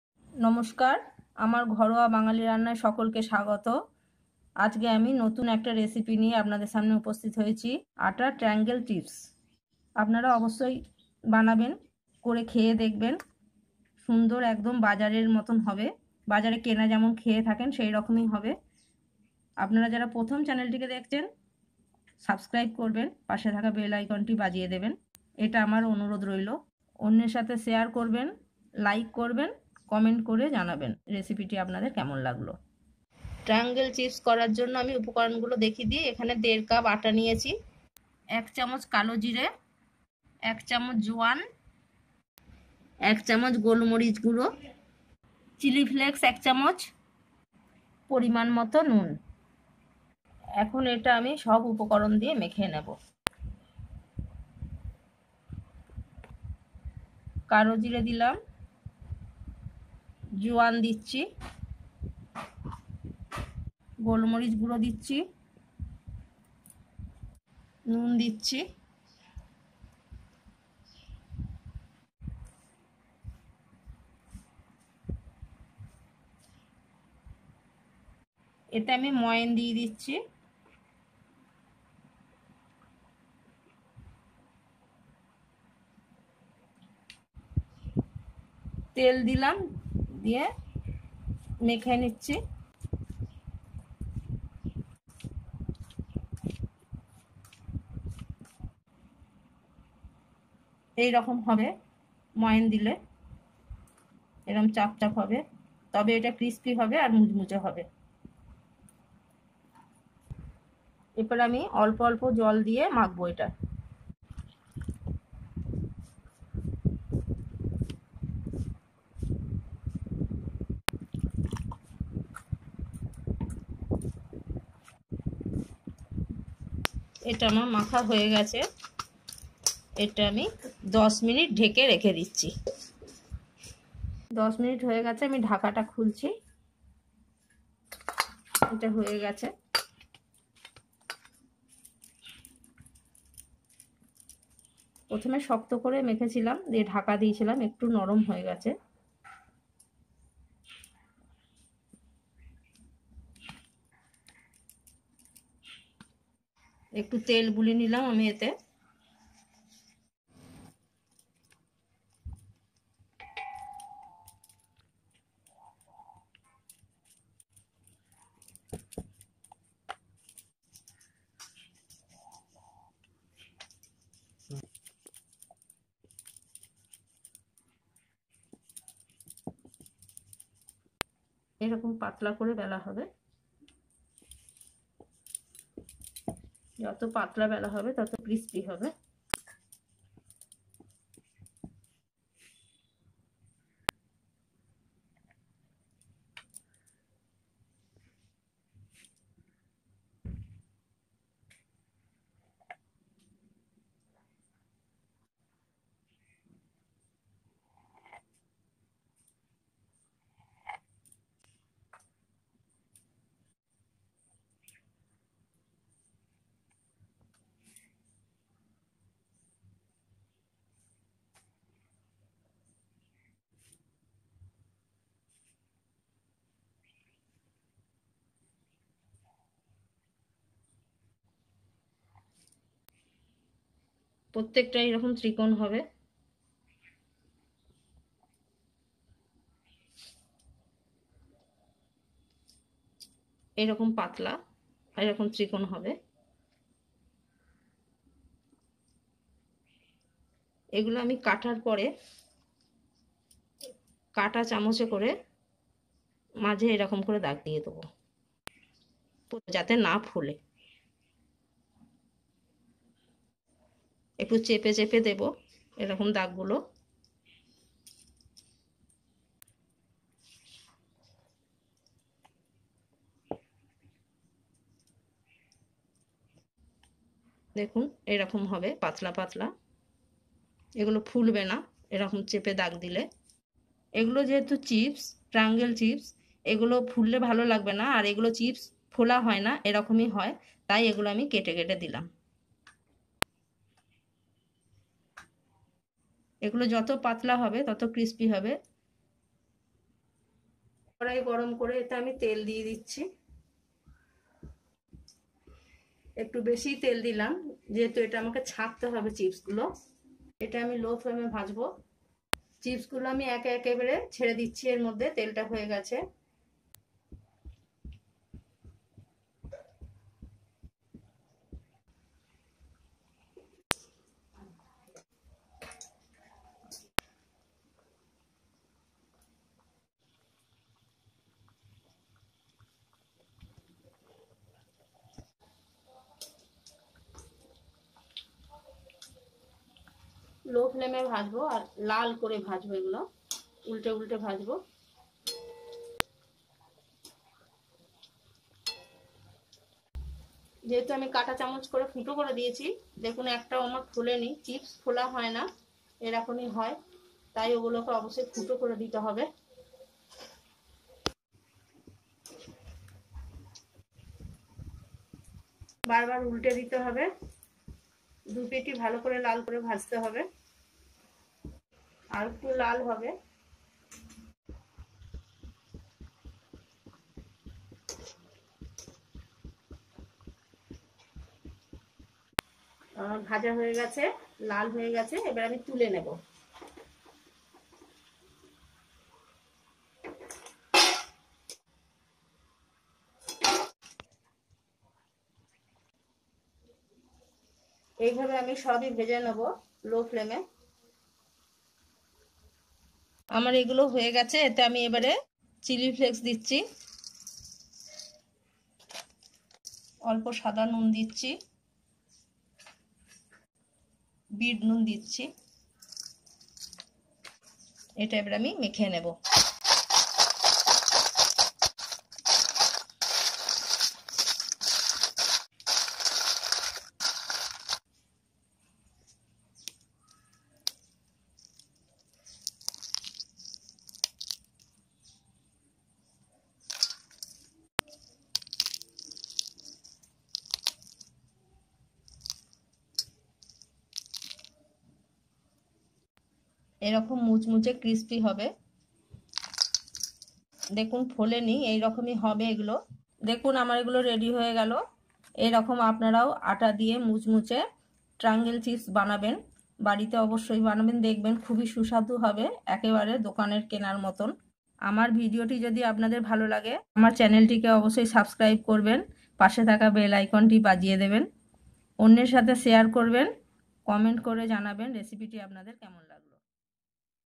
আটার মুচমুচে ট্রায়াঙ্গল চিপসAtta Triangle Chips|Nachos Recipe|Wheat flour chips in BengaliAttarecipe
আটার মুচমুচে ট্রায়াঙ্গল চিপস|Atta Triangle Chips|Nachos Recipe Chips recipe
Atta Recipe Tea time snacks recipeWheat flour chips in bengali Chips recipe in Bengali

Ingredients:
Wheat flour 1½cup
Salt to taste
Black Cumin seeds 1tsp
Black pepper 1tsp
Juan1tsp
Oil
Water as needed
Chili flakes as needed
Rock Salt